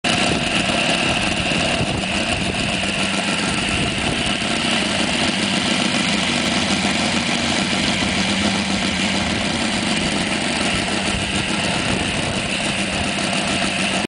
Moteur EVOLUTION Radial 7-77cc au banc
Passage au banc d'un moteur radial EVOLUTION 7-77cc, carburant LABEMA RS Spécial  Radial type Seidel, hélice BIELA tripale Hamilton replica